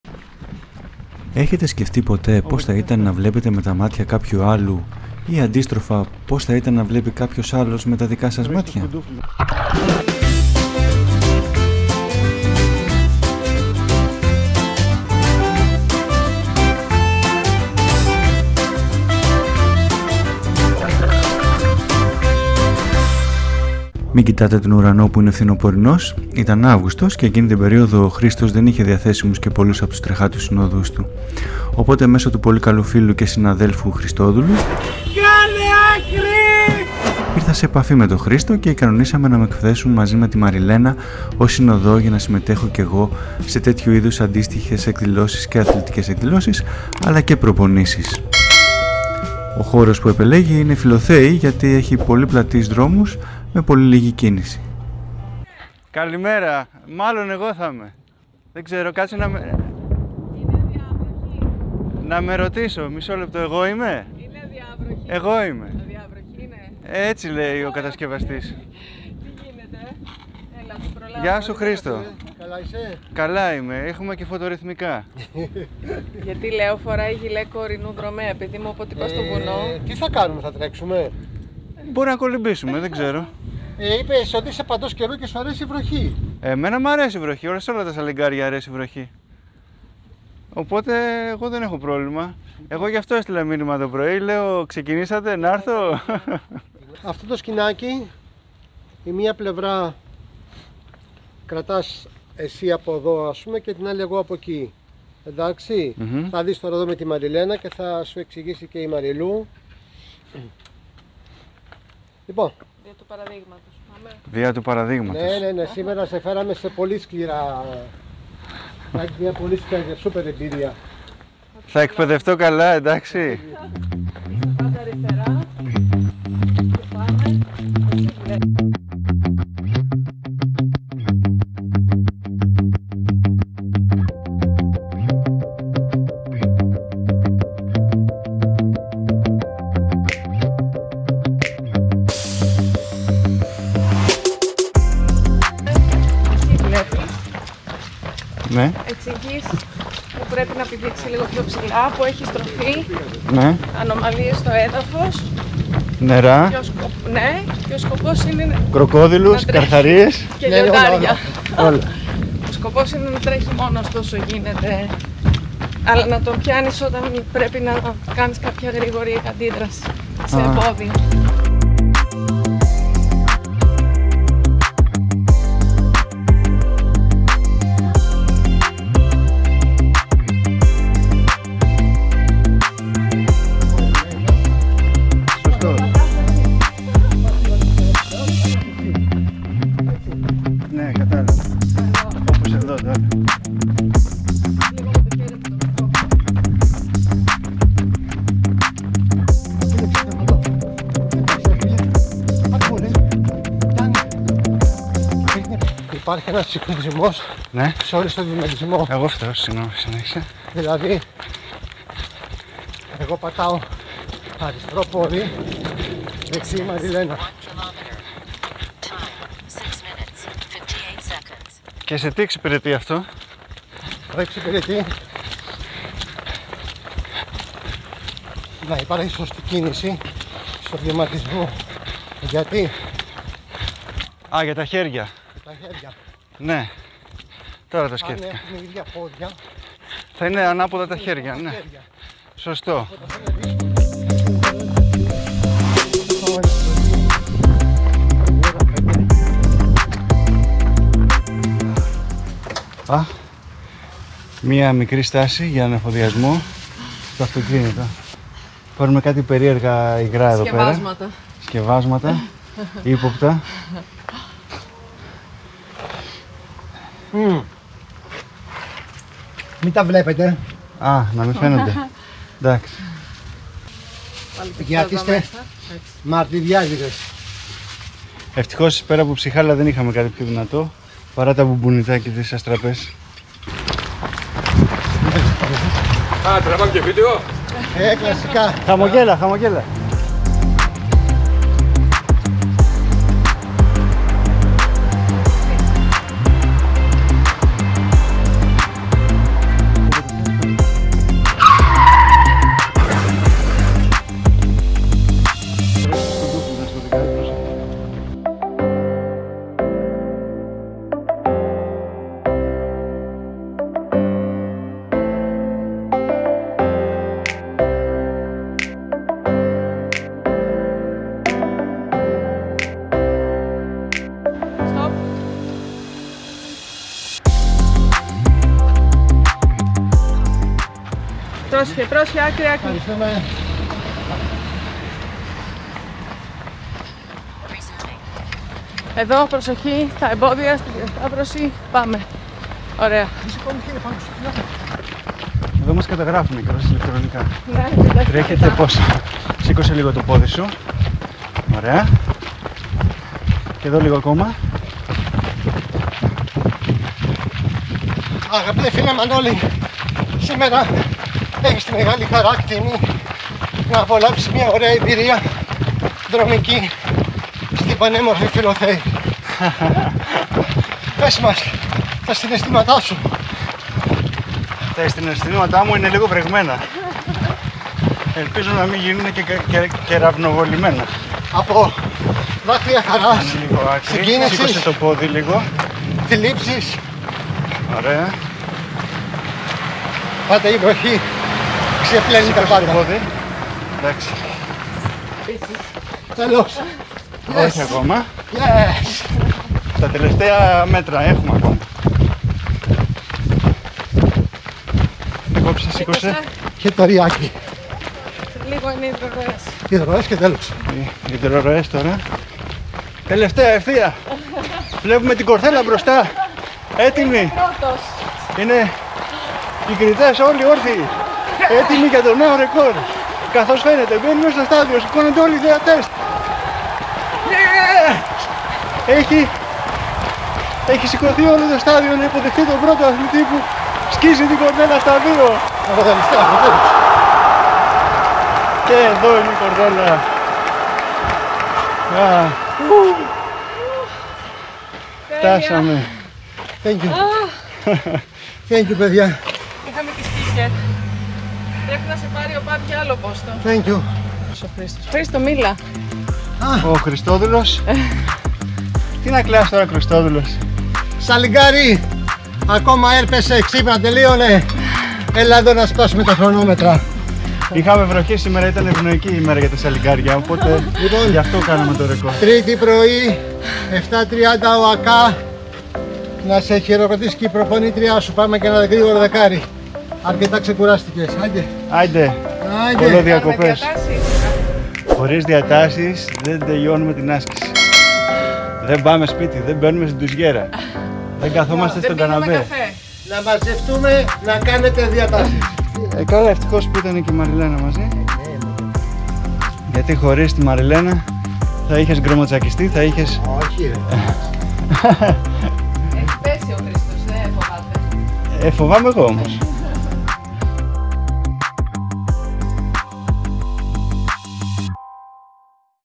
Εκπαιδεύση ως συνοδός δρομέας και τρέξιμο με τον Χρήστο Ζώτο
Ο Χρήστος Ζώτος είναι αθλητής που εδώ και χρόνια έχει χάσει την όρασή του. Αυτό όμως δεν έχει γίνει εμπόδιο στο να προπονείται, να συμμετέχει σε αγώνες και να ζει μια ποιοτική ζωή. Απλώς έχει μια δυσκολία παραπάνω από όλους εμάς τους υπόλοιπους. Η δυσκολία αυτή ξεπερνιέται χάρη στην οικογένειά του και στους φίλους του.

Όταν έμαθα ότι τον Αύγουστο θα χρειαστεί επιπλέον συνοδούς αθλητές για τις προπονήσεις του επειδή κάποιοι θα λείπουν, τότε αμέσως ανταποκρίθηκα. Ευτυχώς δεν ήμουν μόνος γιατί πρώτη φορά χωρίς καθοδήγηση δε θα αισθανόμουν και πολύ άνετα κι έτσι για καλή μου τύχη είχα ως εκπαιδεύτρια τη Μαριλένα χάρη στην οποία έμαθα στην πράξη πώς να καθοδηγώ σωστά.

Ο Χρήστος έχει και δικό του κανάλι στο youtube. Μπορείτε να δείτε τα βίντεό του στο σύνδεσμο